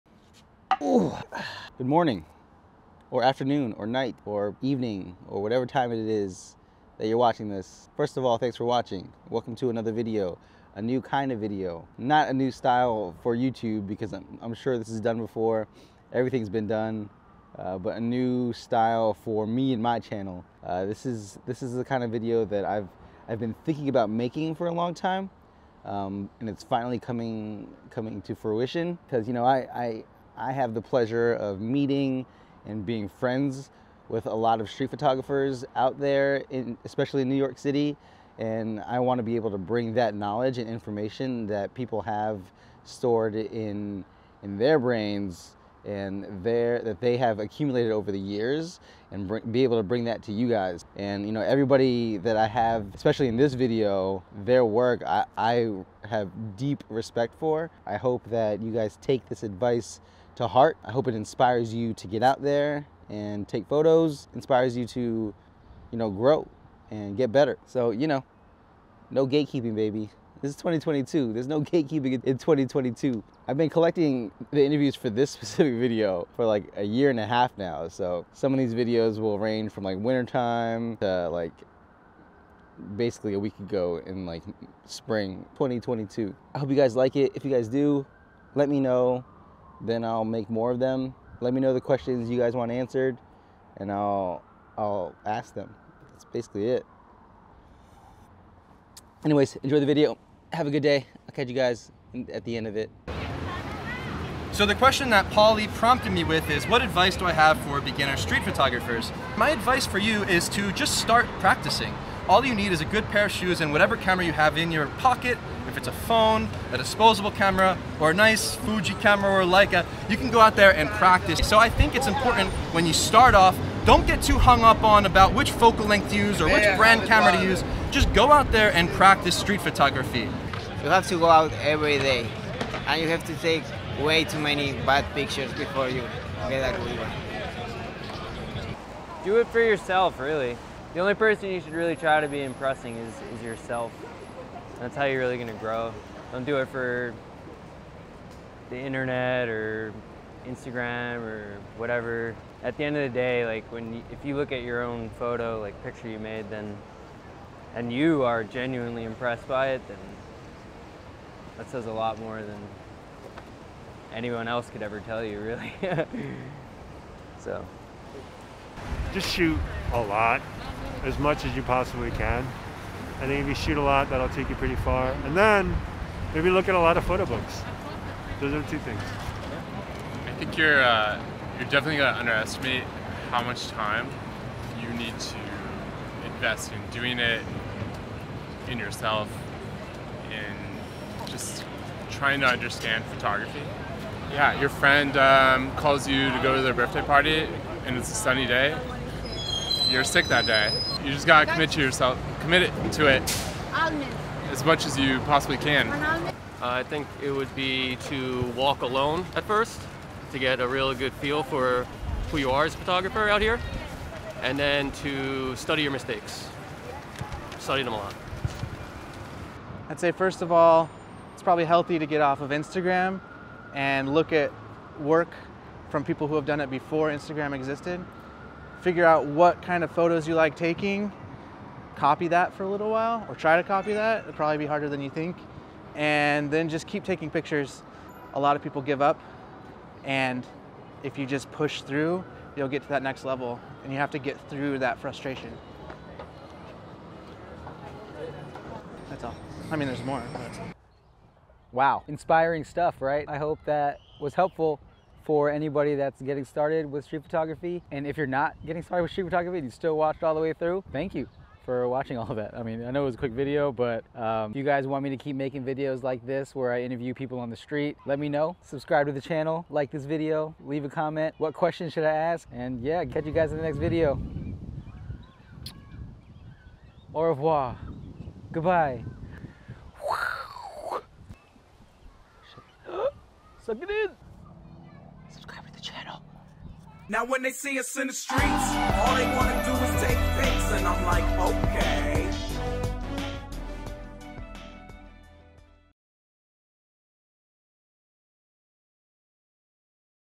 BEGINNER STREET PHOTOGRAPHERS START HERE.
Advice begins at 1:43

Hi. I run into A LOT of photographers while out taking photos in the streets of New York City. In an effort to share knowledge, I'd love to start sharing more from, not just myself, but other photographers that I know and talk to almost every day. Some of which you may know, some of which you may not.

SHARE this with anyone you think would benefit from this or who has recently started in street photography.

Leave a question you think would make a good video similar to this one and I'll consider making it the next video!